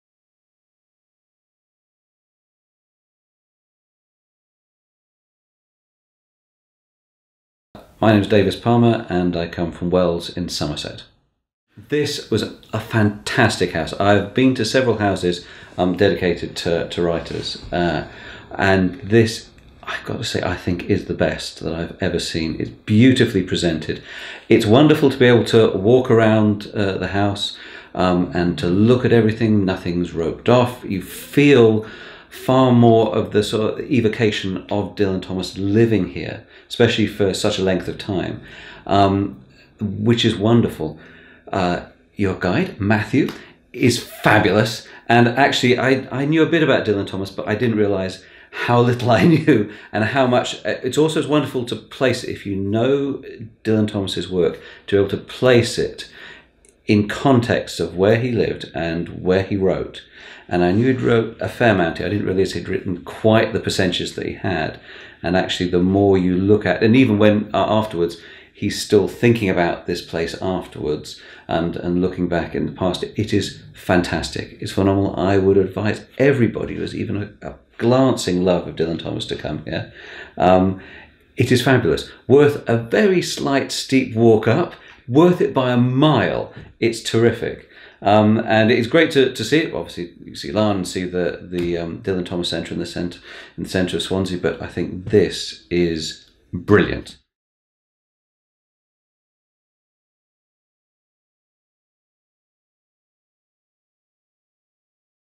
Visitor Review - Davies Palmer (18th May 2017)
Here's a lovely review from Davies Palmer of Wells, Somerset. He's a big fan of Dylan Thomas and is a prolific stage actor. In 2011 he starred in the Pentameters Theatre's production of 'Under Milkwood' playing (First & Second Voice), Captain Cat, Butcher Beynon, Cherry Owen & Lord Cut-Glass.

Thank you Davies!